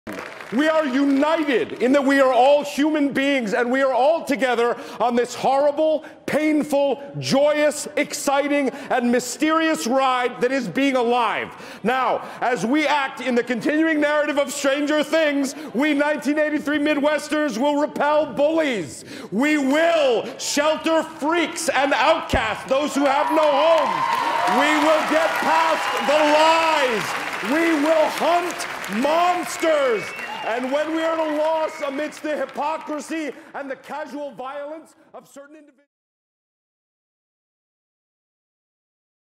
El poderoso discurso del elenco de 'Stranger Things' en favor de "los marginados"
El actor de la serie 'Stranger Things', David Harbour -acompañado del elenco de la serie-, pronunció un discurso donde, sin señalar de manera directa, urgió la necesidad de unidad y solidaridad entre los actores frente a los tiempos que se viven en su país (EU), luego de la llegada de Donald Trump a la presidencia